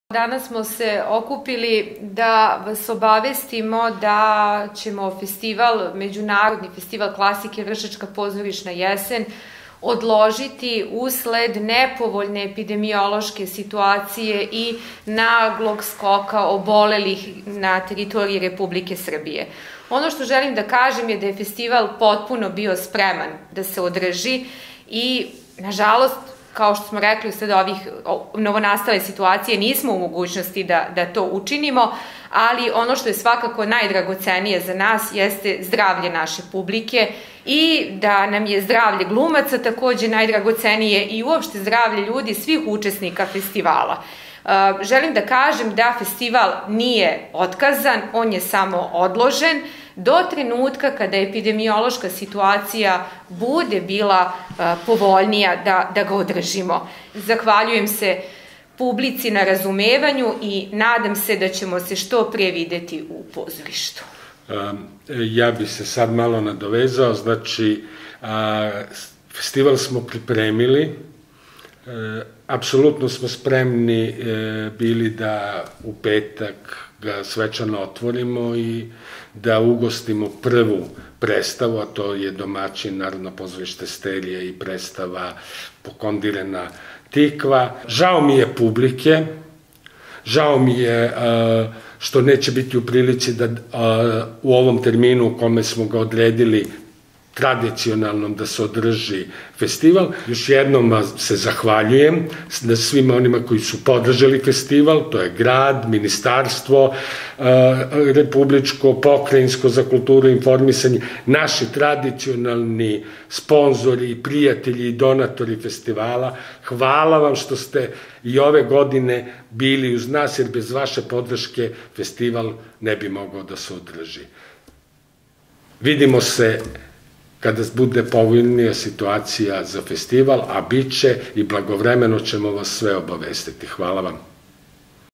Саопштење за медије / Вршачка позоришна јесен
Организациони одбор међународног фестивала класике „Вршачка позоришна јесен“, услед неповољне епидемиолошке ситуације са вирусом COVID-19, а ради очувања здравља публике, глумаца и осталих учесника, донео је одлуку о одлагању овогодишњег фестивала, који је по плану требало да се одржи од 23. октобра до 1. новембра. 28. по реду фестивал „Вршачка позоришна јесен“, у потпуности је био испланиран и припремљен, тако да је његово одржавање померено до тренутка када епидемилошка ситуација у земљи то буде дозвољавала. Организациони одбор фестивала, захваљује се бројној публици на разумевању и одговорности, уз жељу да се што пре видимо у НП „Стерија“ Вршац.